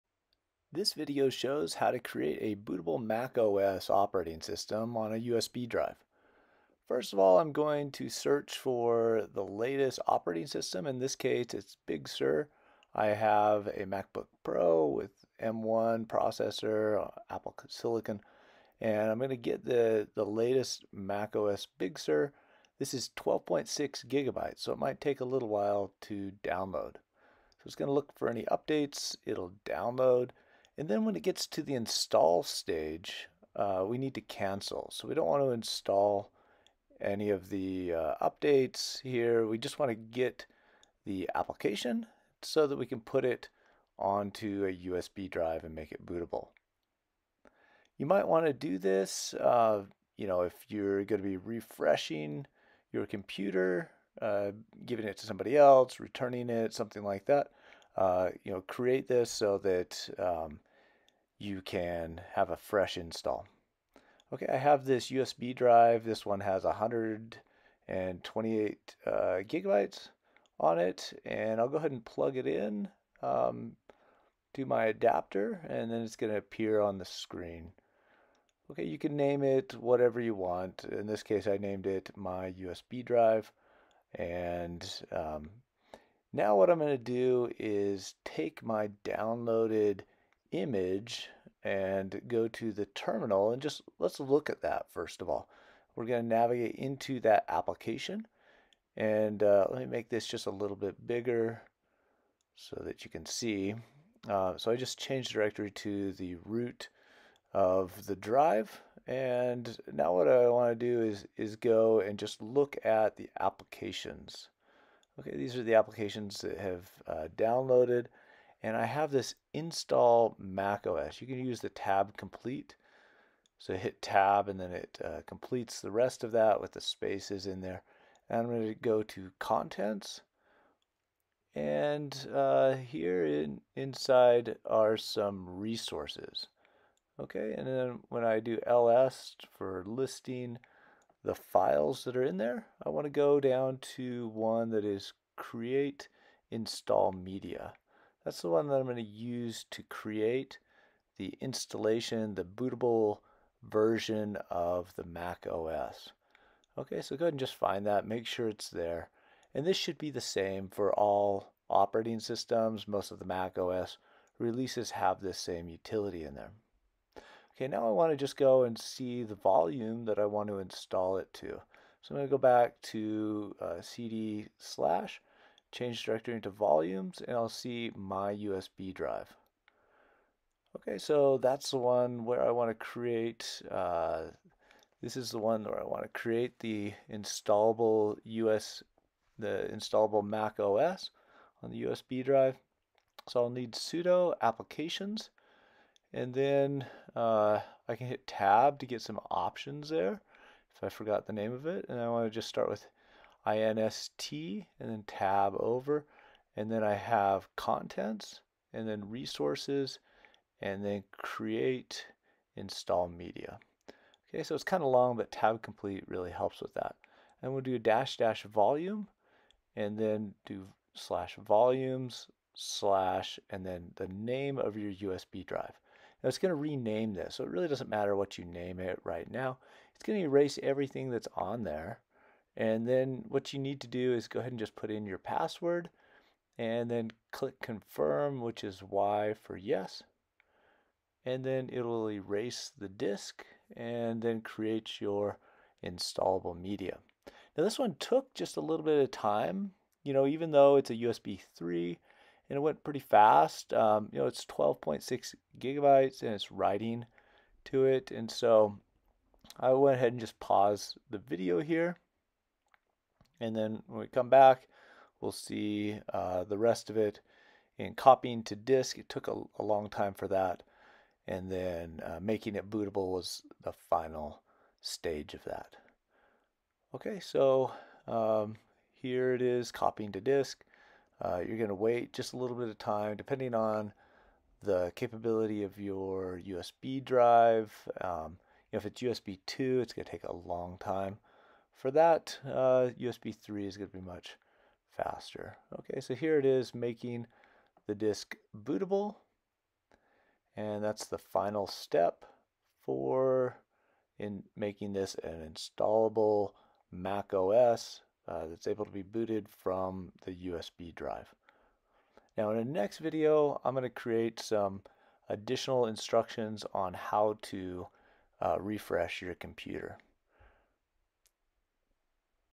Create Bootable MacOS USB for M1 Big Sur
Step-by-step instructions to Download MacOS from Applications store and create a bootable USB drive. A bootable USB drive is needed to reinstall MacOS on a computer if there are no other recovery options.

Option #1: Reset MacOS with USB boot drive as backup
power button when starting to enter utility mode

Create bootable drive with at least 12.6 GB USB drive
sudo /Applications/Install\ macOS\ Big\ Sur.app/Contents/Resources/createinstallmedia --volume /Volumes/My_USB_Drive

There is currently a bug in reseting the MacOS that says the user account cannot be configured. You can type command "resetpassword" from the recovery terminal and select option to Erase Mac hard drive completely. Once this is complete, the reinstall from a USB drive or Recovery partition is successful.

Option #2: Restore from another computer
Connect MacOS (Catalina+ that has configurator 2) computer to MacBook Pro / Air M1
1. Get a USB cable and connect together. It is USB-C back left port for the M1 and USB-C or USB-A for older MacOS to perform the restore.
2. Download and open Configurator 2 on the other Mac
3. Enter DFU mode on the M1 (hold Left-Ctrl + Left-Option + Right SHIFT + Power button) until it pops up in Configurator 2. Hold for long enough, typically at least 15 seconds.
4. In Configurator, right-click on the icon. In the menu, click Advanced…Restore to erase everything.
5. Download Big Sur (12 GB) and send it to the M1. Afterwards, you'll see the factory setup screen.